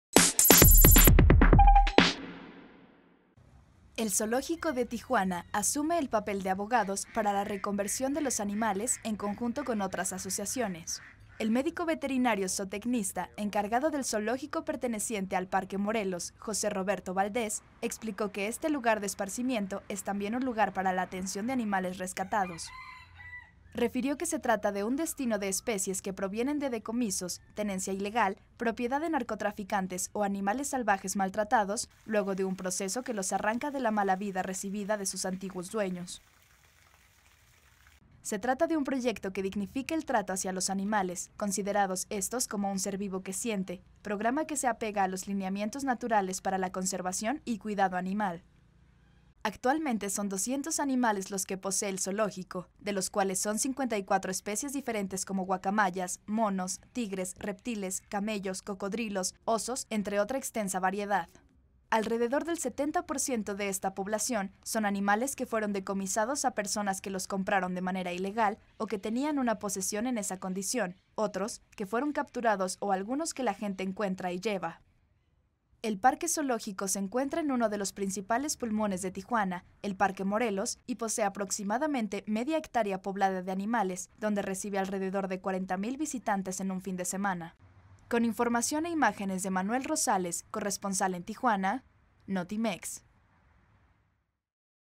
Zoológico de Tijuana es también santuario de animales rescatados
El Parque Zoológico se encuentra en uno de los principales pulmones de Tijuana, y recibe alrededor de 40 mil visitantes en un fin de semana